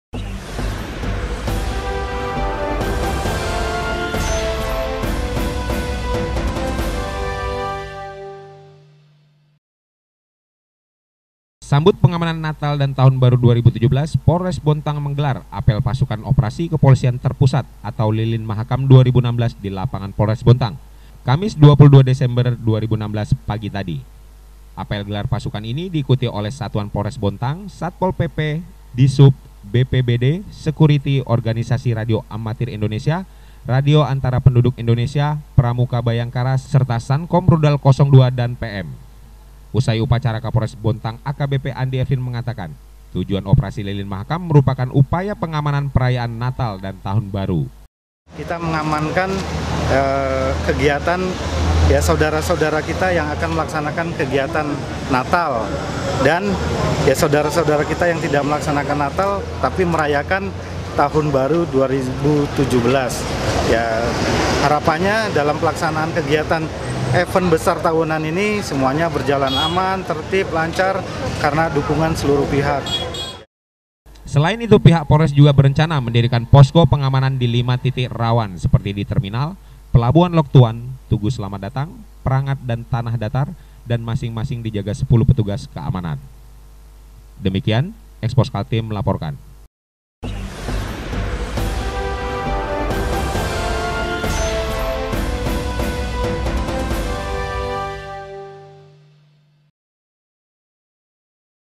POLRES BONTANG GELAR APEL OPERASI LILIN MAHAKAM 2016
SAMBUT PENGAMANAN NATAL DAN TAHUN BARU 2017, POLRES BONTANG MENGGELAR APEL PASUKAN OPERASI KEPOLISIAN TERPUSAT ATAU LILIN MAHAKAM 2016 DI LAPANGAN POLRES BONTANG, KAMIS 22 DESEMBER 2016.

REPORTER : ENDAR